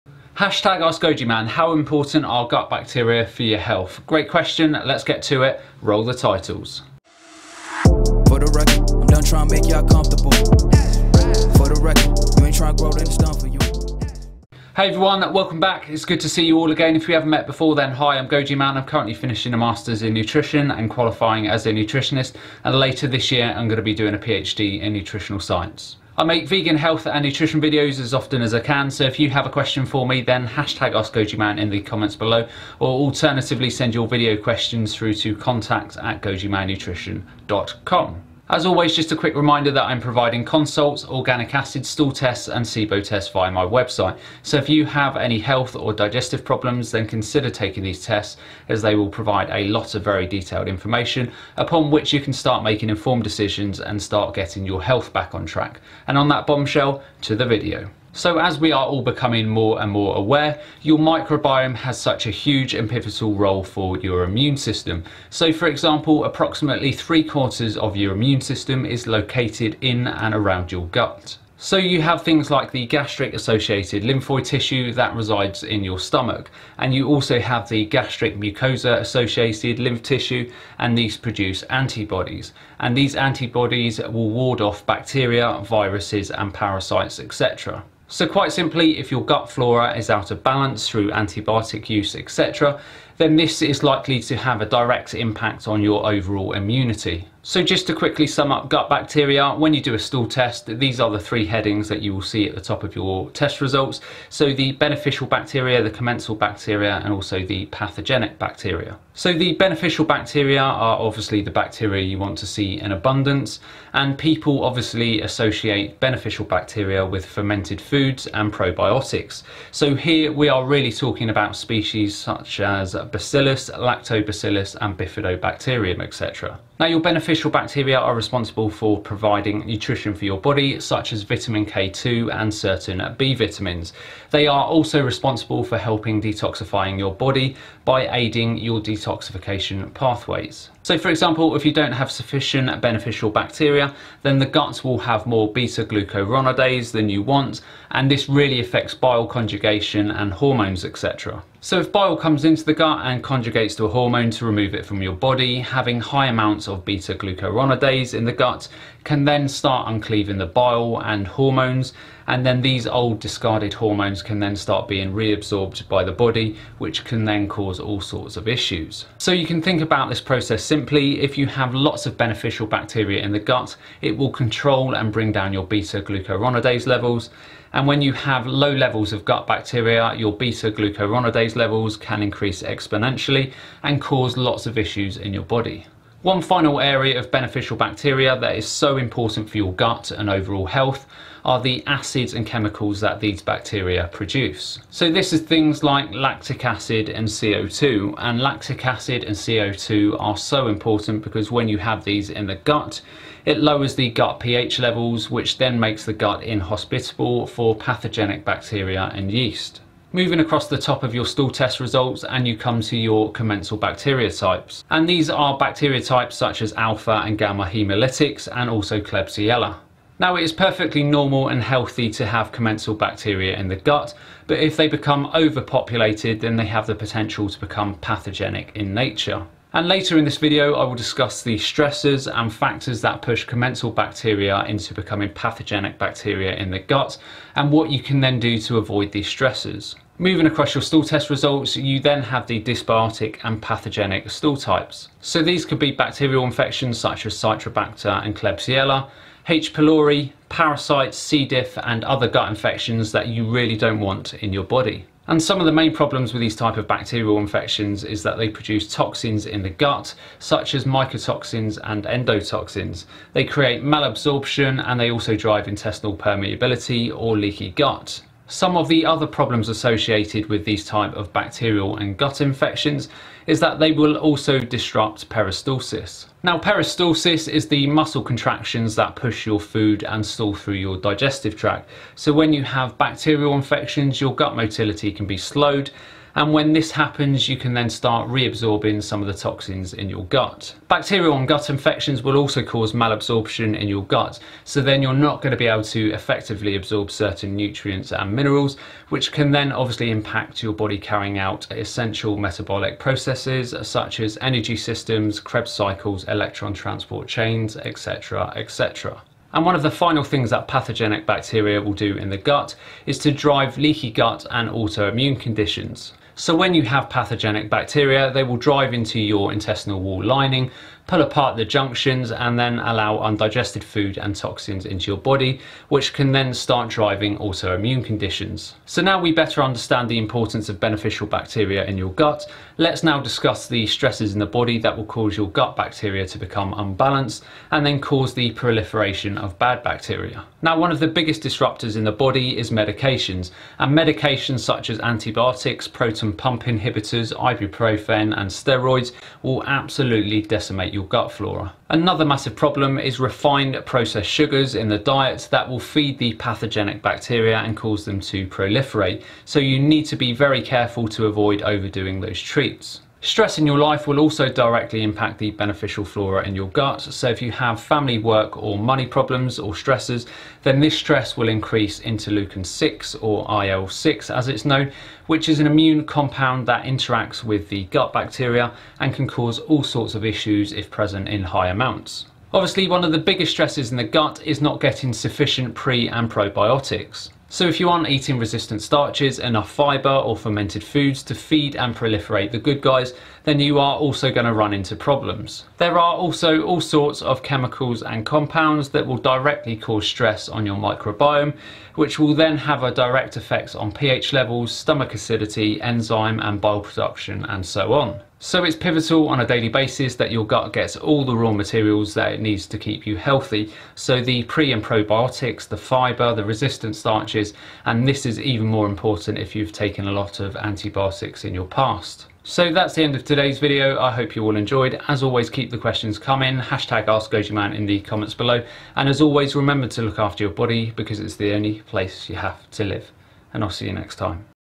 How Gut Bacteria Affects Your Health
In todays video I discuss how Gut Bacteria Affects Your Health.

As always if you have a question for me then askgojiman in the comments below or send your video question through to